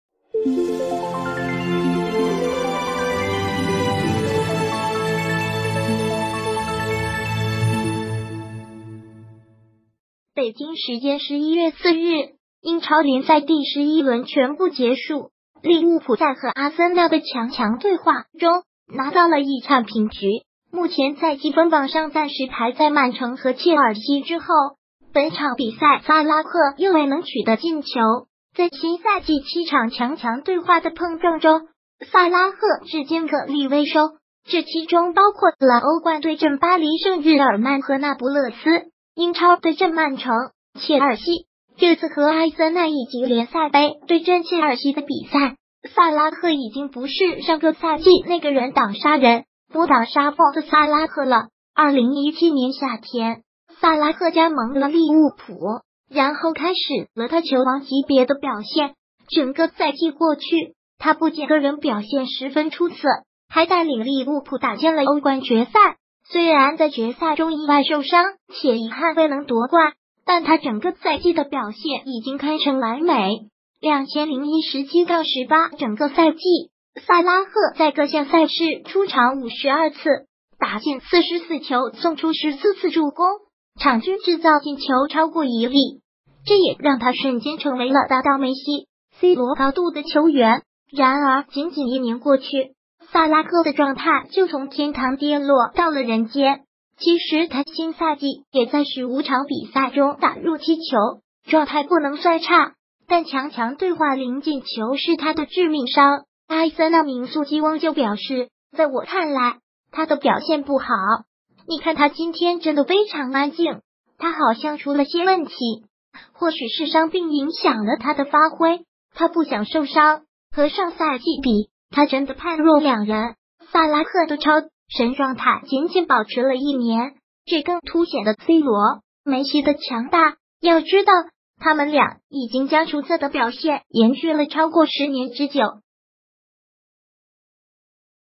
强强对话7场0球！新赛季萨拉赫跌落人间，更显梅罗的强大
北京时间11月4日，英超联赛第11轮全部结束，利物浦在和阿森纳的强强对话中拿到了一场平局，目前在积分榜上暂时排在曼城和切尔西之后。本场比赛萨拉赫又未能取得进球，在新赛季7场强强对话的碰撞中，萨拉赫至今颗粒未收。这其中包括了欧冠对阵巴黎圣日耳曼和那不勒斯，英超对阵曼城、切尔西、热刺和阿森纳以及联赛杯对阵切尔西的比赛，萨拉赫已经不是上个赛季那个人挡杀人、佛挡杀佛的萨拉赫了！2017年夏天，萨拉赫加盟了利物浦，然后开始了他球王级别的表现。整个赛季过去，他不仅个人表现十分出色，还带领利物浦打进了欧冠决赛，虽然在决赛中意外受伤且遗憾未能夺冠，但他整个赛季的表现已经堪称完美。2017-18整个赛季，萨拉赫在各项赛事出场52次，打进44球送出14次助攻，场均制造进球超过1粒，这也让他瞬间成为了达到梅西、C罗高度的球员。然而仅仅一年过去，萨拉赫的状态就从天堂跌落到了人间，其实他新赛季也在15场比赛中打入7球，状态不能算差，但强强对话0进球是他的致命伤。阿森纳名宿基翁就表示：“在我看来，他的表现不好。你看他今天真的非常安静。他好像出了些问题。”“或许是伤病影响了他的发挥，他不想受伤。（和上赛季比）他真的